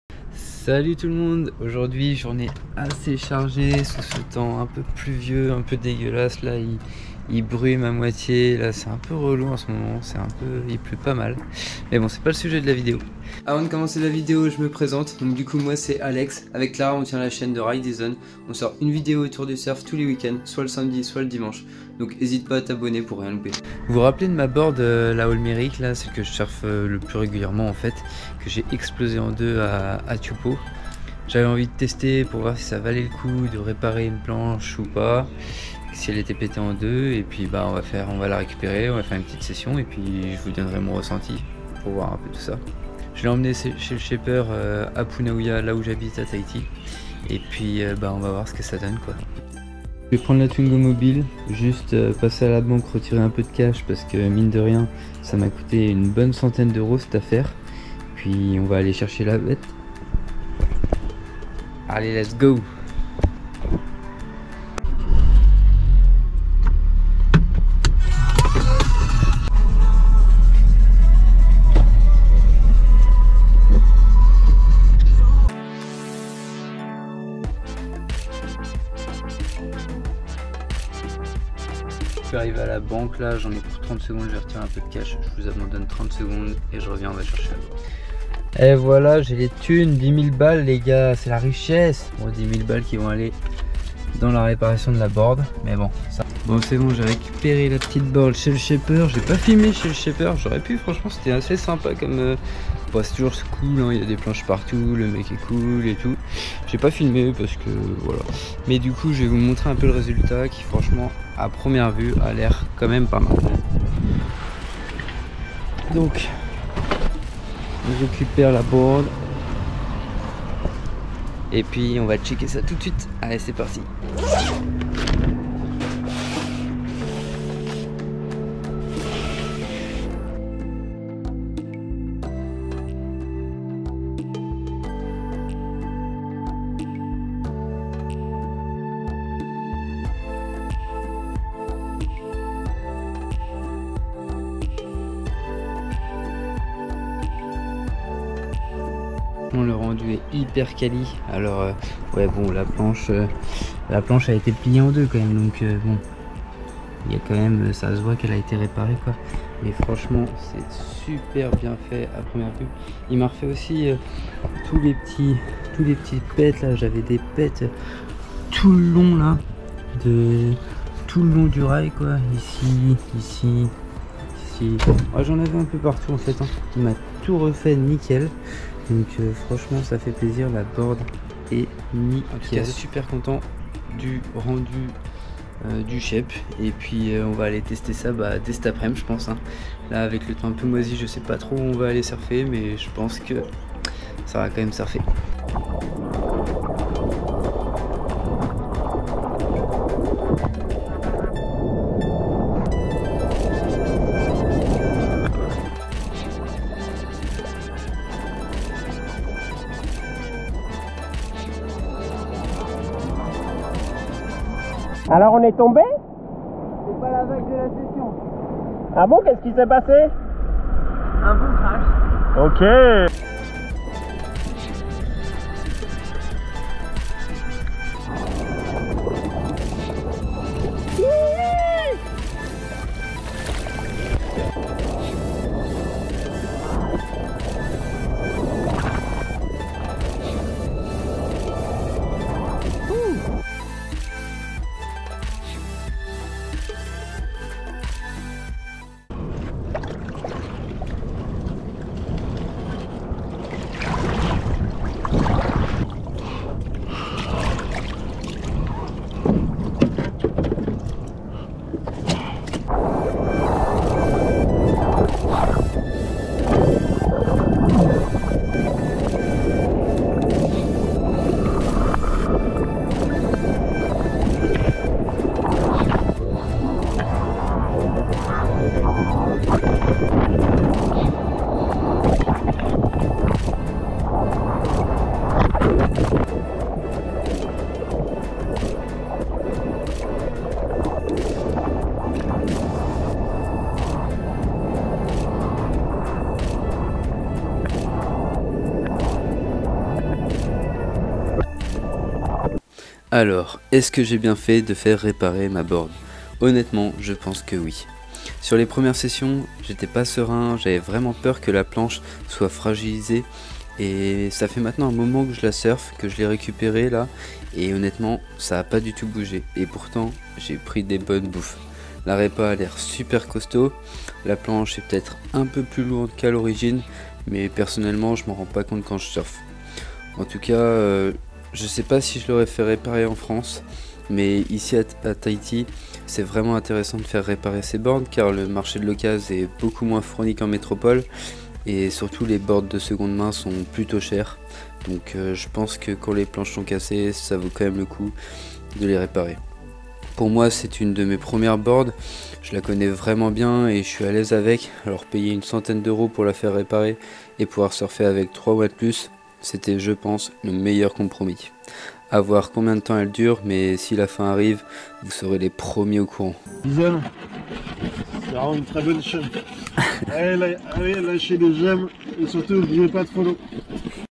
Faut il réparer une planche cassée en deux ? (test et retour)
Salut tout le monde, aujourd'hui j'emmene ma board chez le shaper ! on va voir si ca vaut vraiment le coup de faire réparer une board cassée en deux. donc on regarde tout ça je la surf, et je vous donne mon ressenti !